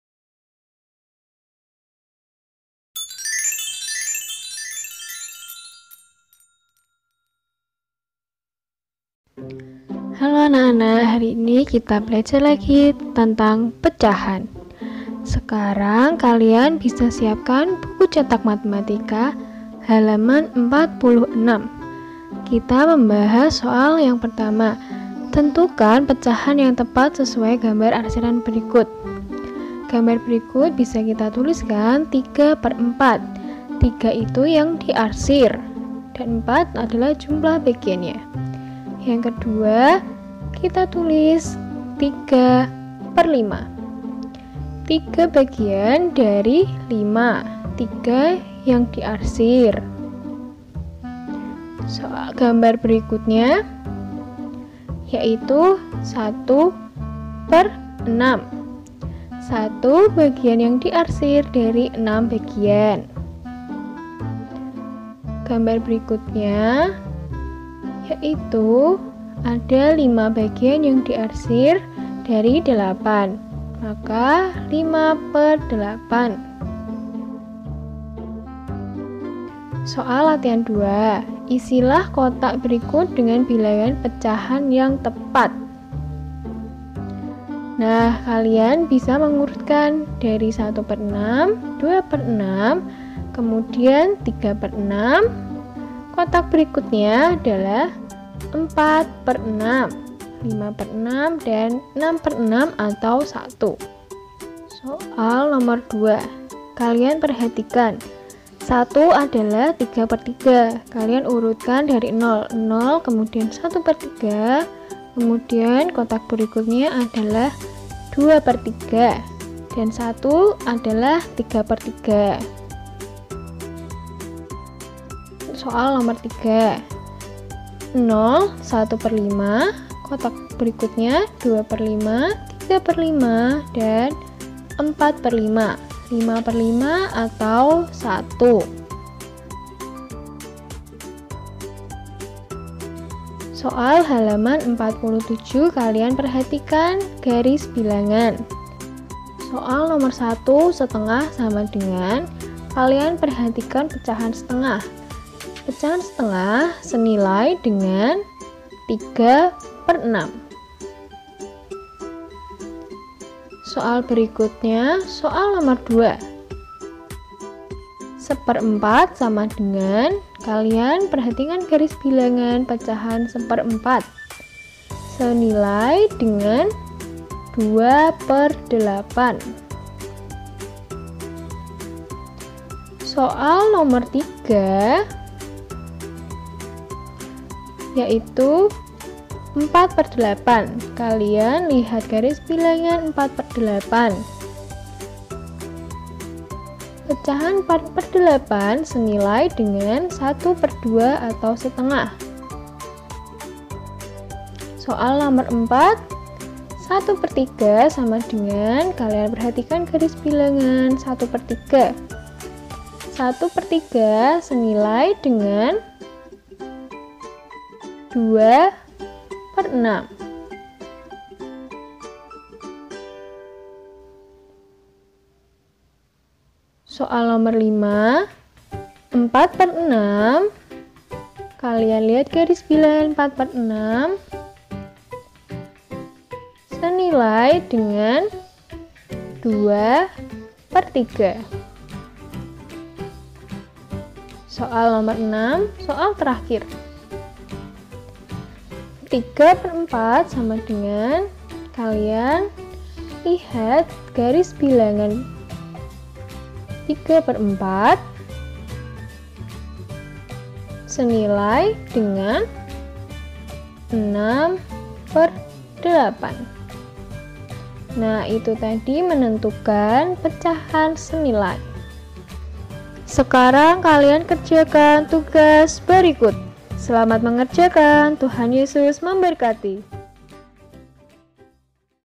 Pecahan l Matematika Kelas 3 Semester Ganjil
MTK Kelas 3
Pecahan
KD. 3.1  menjelaskan sifat-sifat operasi hitung pada bilangan cacah

Syalom anak-anak kelas 3.
Anak-anak pernah membelah semangka menjadi 2 bagian? Menjadi 4 bagian? Dan seterusnyaa..
Mari kita belajar bersama tentang pecahan.
Semangat belajar.
Tuhan memberkati

video ini adalah salah satu rangkaian video pembelajaran yang dibuat oleh guru-guru SD Masehi Kudus untuk peserta didik belajar di rumah pada saat pandemi covid-19.